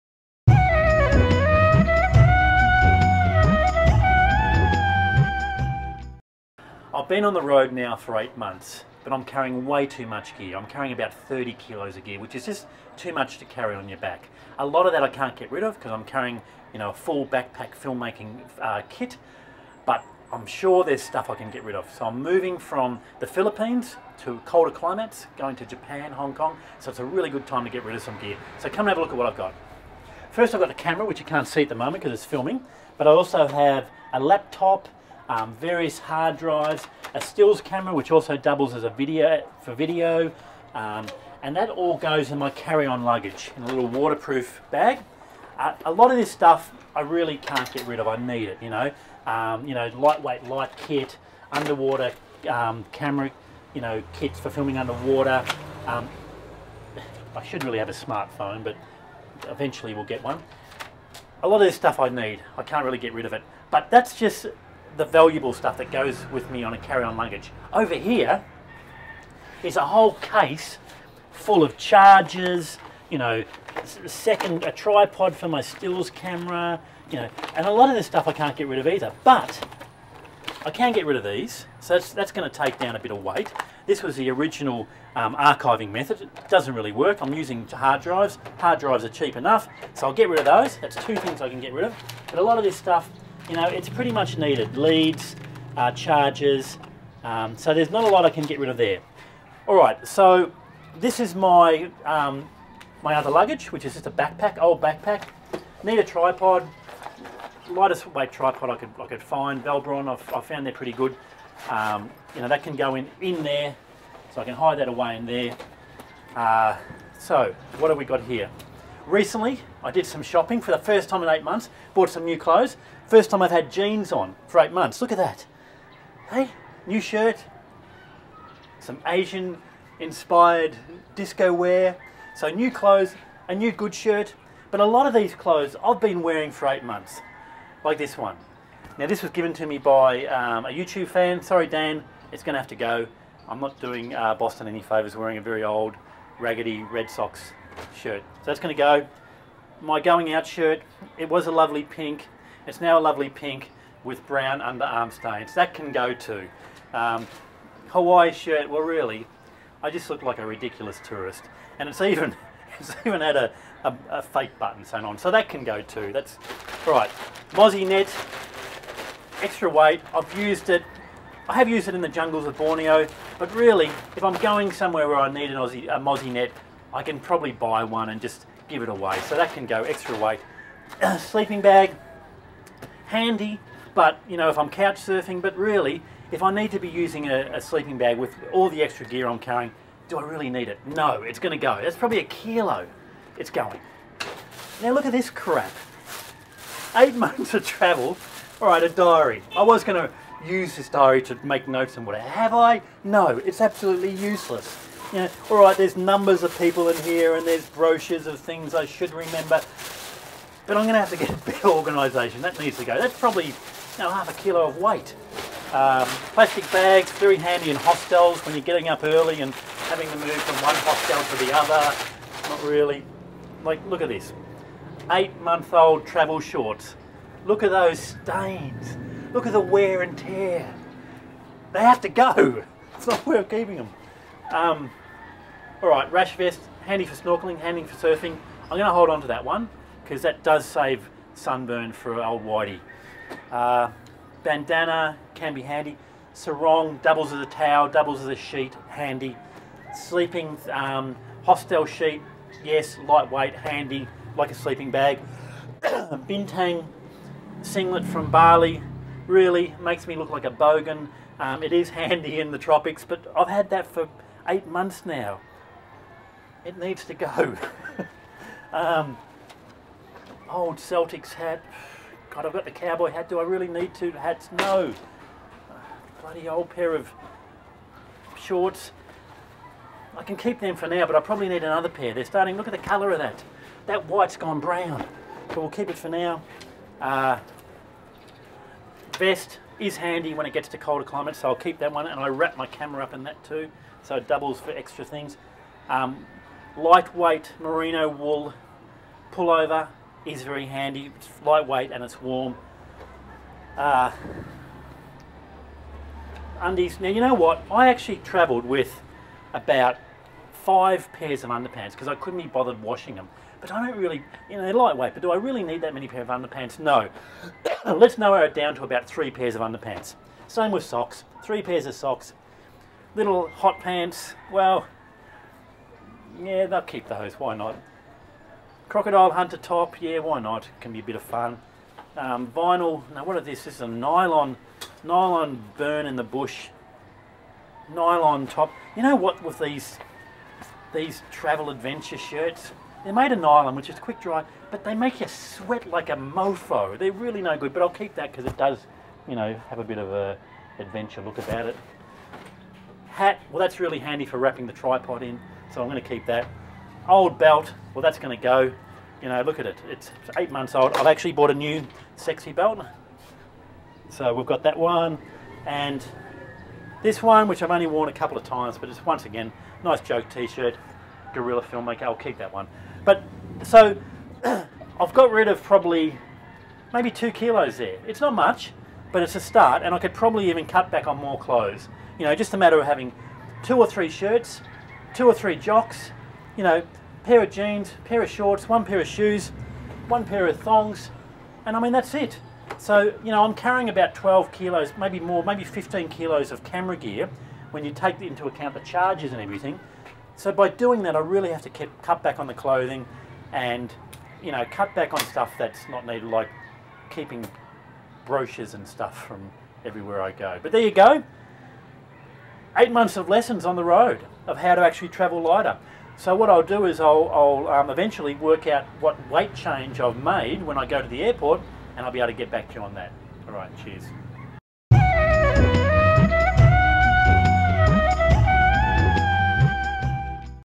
Guide to packing for long term travel
After eight months on the road I decide to try and cut down my 30 kg's of gear. As a backpack filmmaker I carry about 15kg's of camera gear. computer and hard drives.